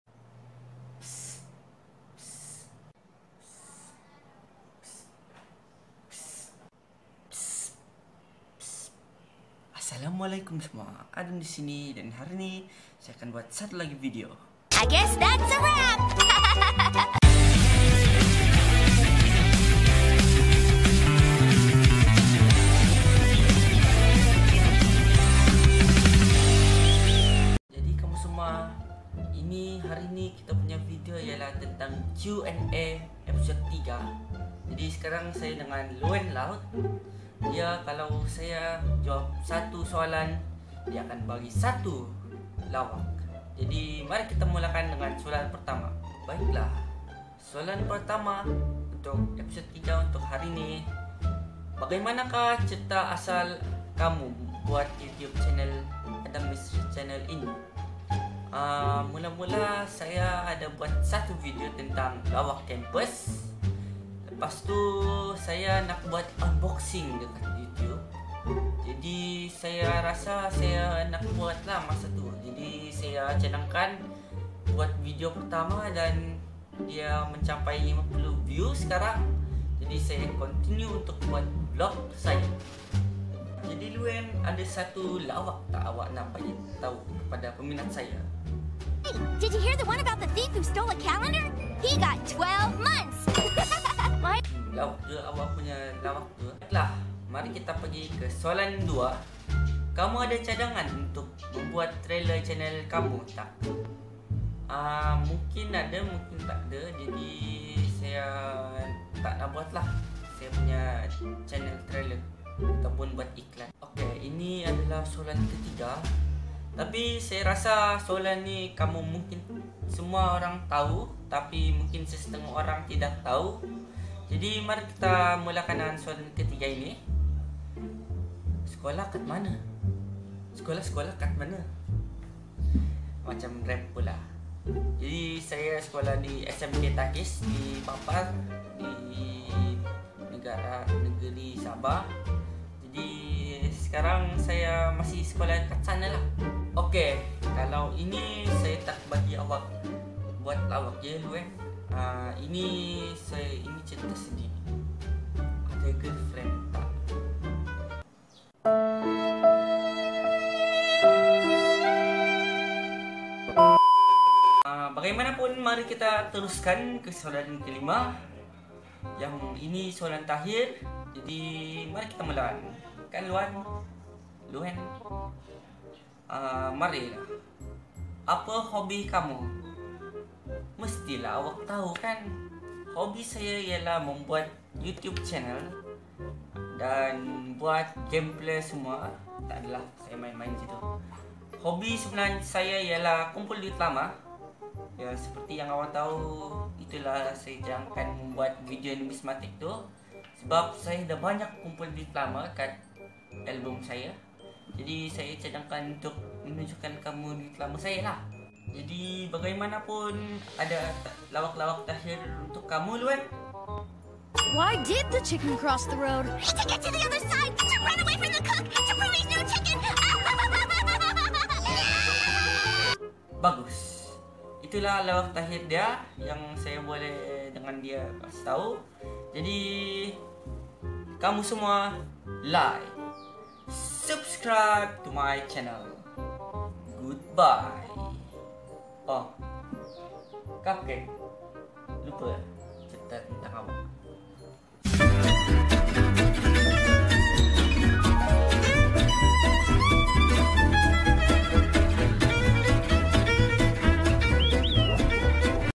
Q&A Episode 3:Question And Comedy (with Luan Loud and The Good Advice Cupcake)
Today Im making another Q&A Episode Series and Im with the Troublemakers Duo and make it trio (Me)

But whatever,Like and Subscibe to My and her channel.